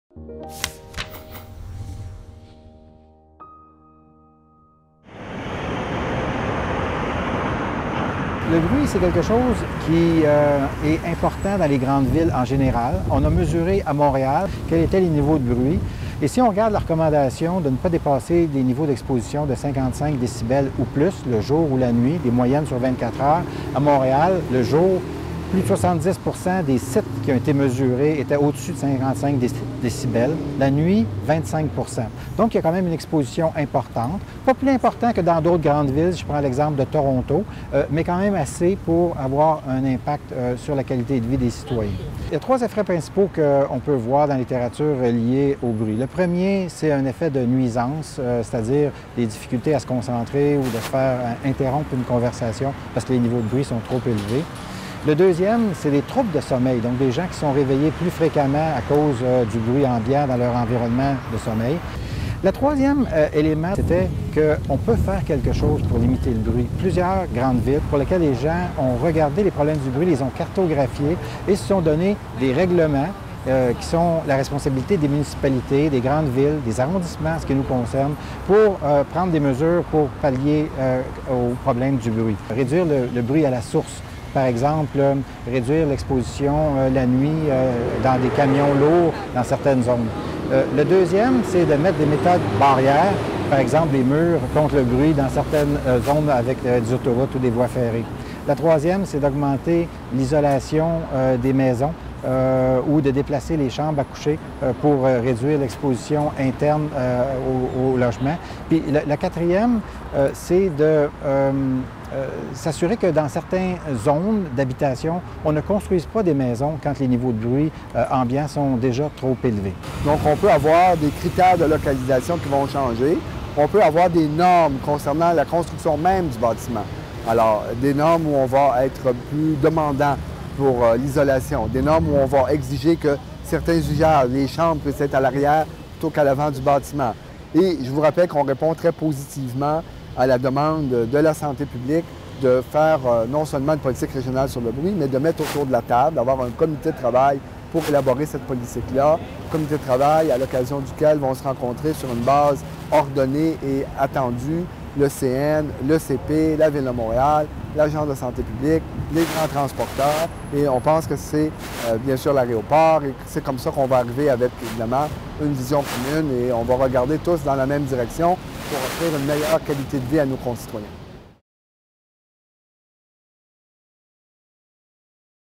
Le bruit du transport - impacts potentiels sur la santé
Le directeur de santé publique de Montréal, Dr Massé, se réjouit de pouvoir travailler avec la Ville de Montréal afin de prévenir les impacts potentiels du bruit sur la santé des Montréalais